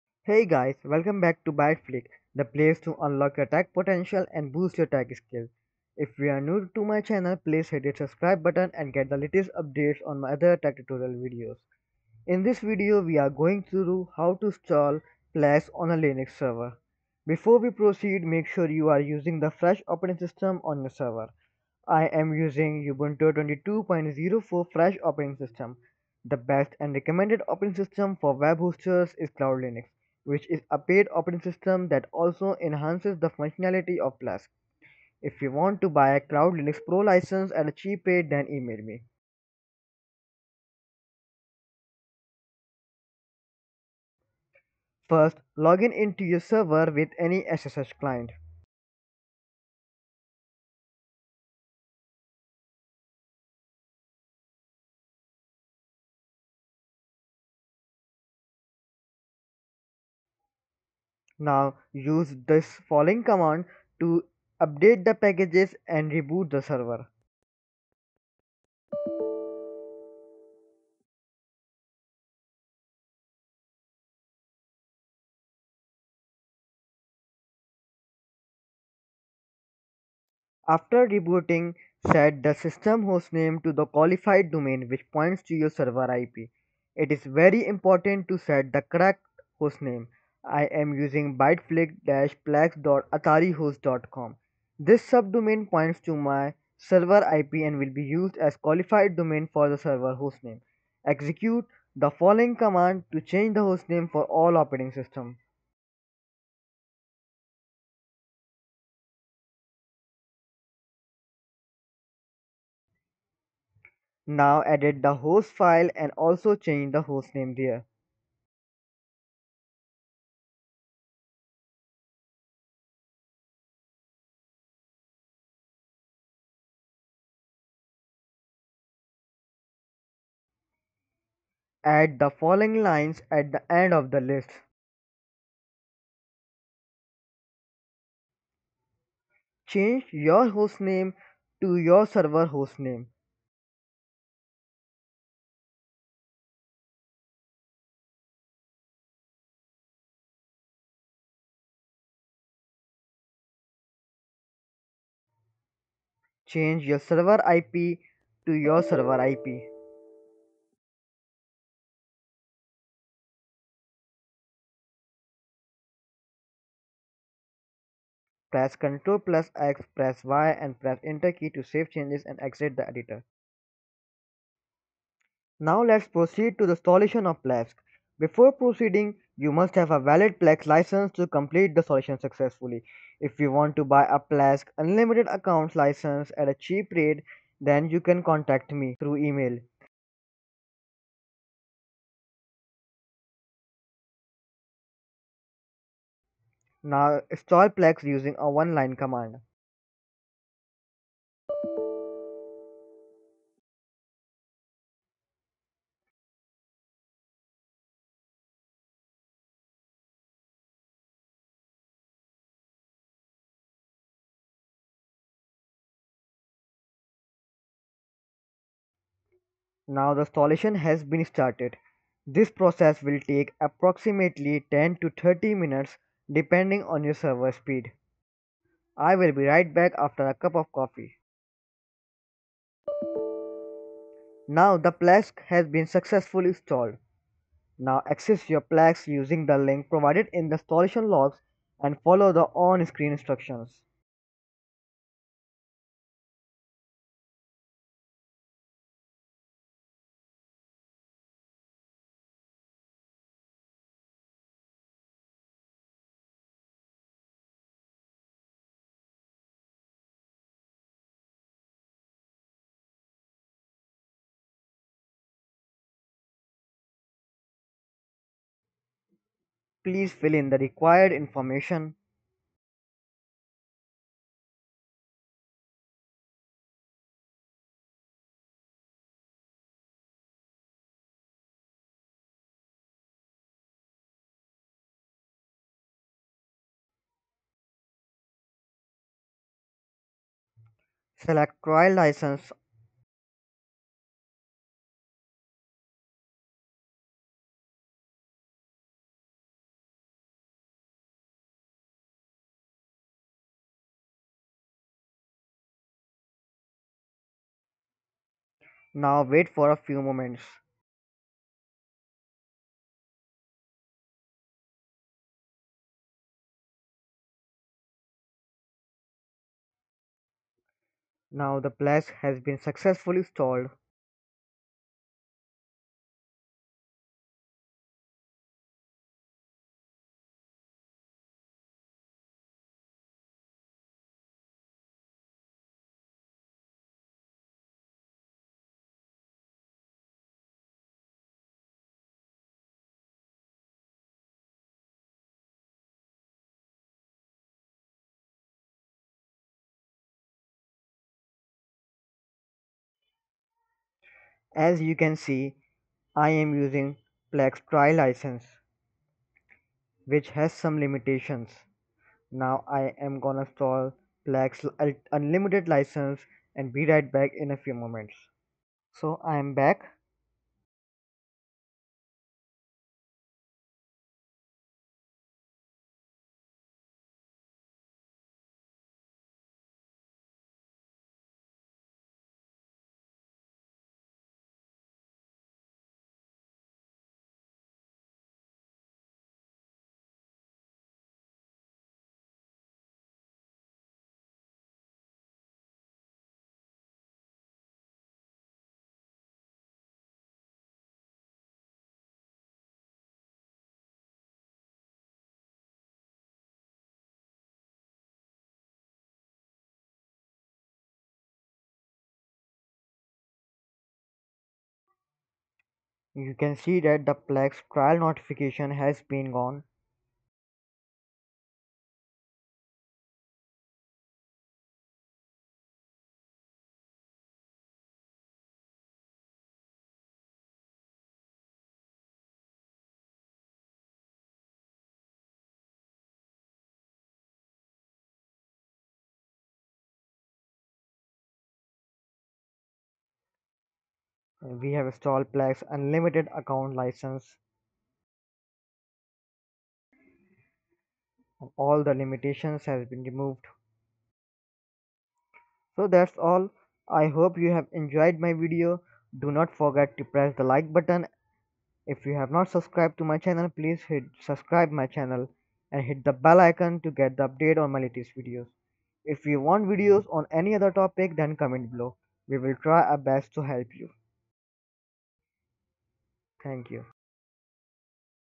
Install Plesk Panel on Linux | Ubuntu | Centos | Free Installation | Web Hosting
Video Chapters:
00:00 Intro
00:45 Update and Reboot Server
01:21 Change Server Hostname
02:56 Install Plesk
03:55 Accessing Plesk
05:55 License Installation
7:35 conclusion

Hey Guys! Welcome back to Byte Flick the place to unlock your tech potential and boost your tech skills.

In this video, we are going through how to install Plesk on a Linux server.

Before we proceed, make sure you are using the fresh Operating System on your server.

I am using the Ubuntu 22.04 fresh operating system. The best and recommended Operating system for web hosters is Cloudlinux which is a paid operating system that also enhances the functionality of Plesk.

First, log into your server with any SSH client then update the packages and reboot the server.

Ubuntu/Debian
sudo apt update -y && sudo apt upgrade -y && sudo reboot

Centos/Cloudlinux/AlmaLinux/Rocky Linux
sudo yum update -y && sudo yum upgrade -y && sudo reboot

After rebooting, set the system hostname to the qualified domain which points to your server IP. It is very important to set the correct hostname.

This subdomain points to my server IP and will be used as a qualified domain for the server hostname. Execute the following command to change the hostname for all operating systems.

sudo hostnamectl set-hostname yourhostname

Now edit the hosts file and also change the hostname there.

Ubuntu/Debian
sudo apt install nano -y

Centos/Cloudlinux/AlmaLinux/Rocky Linux
sudo yum install nano -y

sudo nano /etc/hosts

Add the following lines at the end of the list.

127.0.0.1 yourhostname
serverip yourhostname

Press CTRL + X and press Y and press enter to save changes and exit the editor

Now let's proceed to the installation of Plesk. Before proceeding you must have a valid Plesk license to complete the installation successfully.

wget -O installer.sh && sh installer.sh

This process will take approximately 10 - 30 minutes depending on your server speed.

Now access your Plesk using the link provided in the installation logs and follow the on-screen instructions.

Please fill in the required information. Now wait for a few minutes.

I will be right back after installing the license. As you can see I have installed the unlimited accounts license.

Congratulations! We have successfully installed the Plesk on the Linux server. So, that's all. I hope you have enjoyed my video. Do not forget to press the Like button.